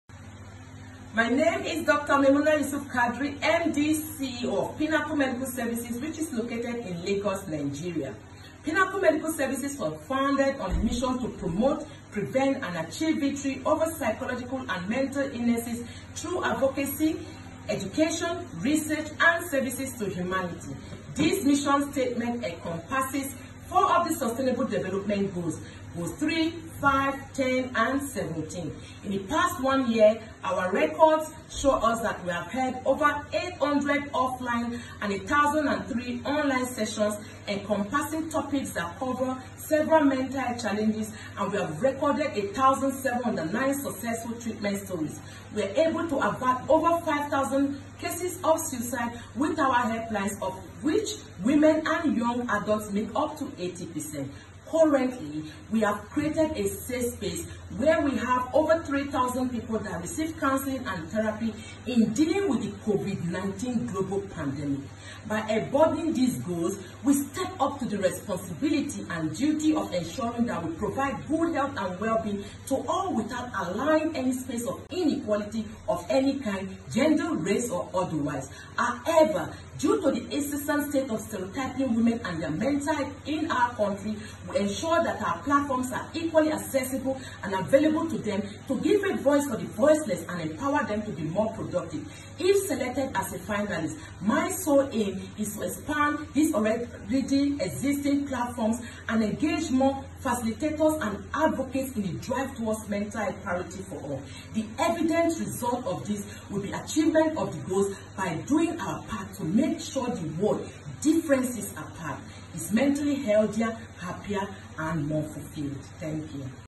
The WE Empower UN SDG Challenge 2020
Dr. Maymunah Yusuf Kadiri advocating for mental health in Nigeria